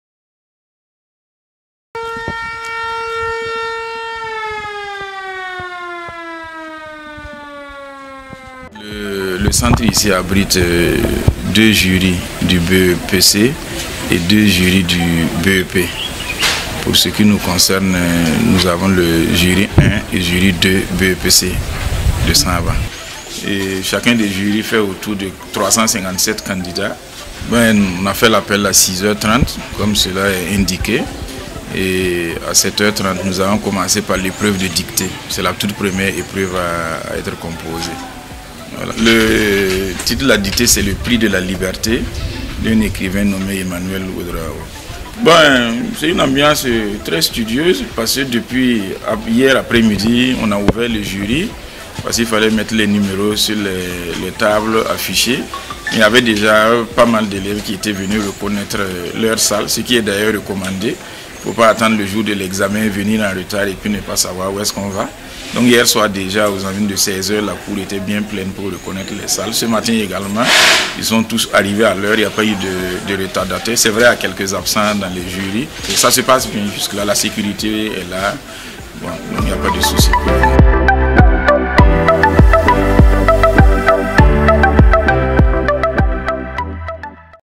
Burkina : Début des examens scolaires ce 04 Juin 2024.
Au Gabriel Taborin où se déroulent les compostions, nous y étions pour soutenir les candidats. Tout se déroule bien selon Dieudonné Noël BAGUE, président du Jury N°1 de SAABA.